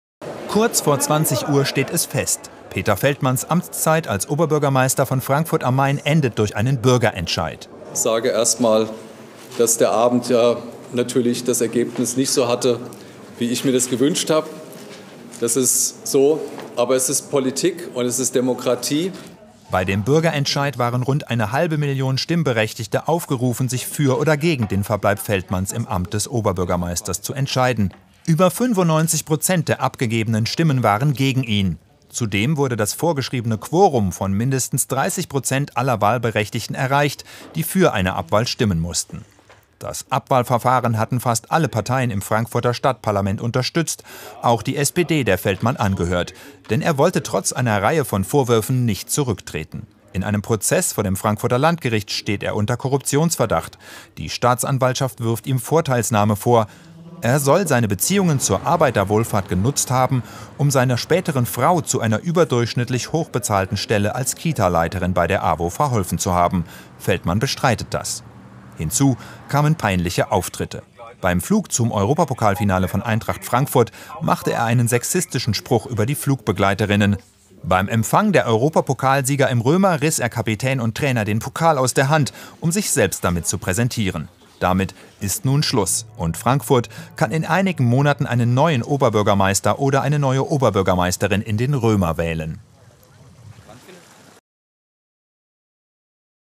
SPD Obm. von Ffm. Feldmann durch Bürgerentscheid abgewählt
Quorum bei Bürgerentscheid erreicht - Oberbürgermeister Feldmann ist abgewählt

Der umstrittene Oberbürgermeister von Frankfurt am Main, Feldmann, ist abgewählt.
In einem Bürgerentscheid wurde die nötige Stimmanzahl deutlich übertroffen. Feldmann steht derzeit wegen Vorteilsnahme vor Gericht.

Peter Feldmann ist als Oberbürgermeister von Frankfurt abgewählt. Das nötige Quorum von 30 Prozent aller Wahlberechtigten wurde bei dem Bürgerentscheid erreicht. Der SPD-Politiker trat noch vor Ende der Auszählung vor die Presse und räumte seine Abwahl ein.
"Das heißt, dass ich damit dann am Freitag kein Oberbürgermeister mehr sein werde", sagte er. Formal ist er noch bis Ende der Woche Chef der Stadtverwaltung.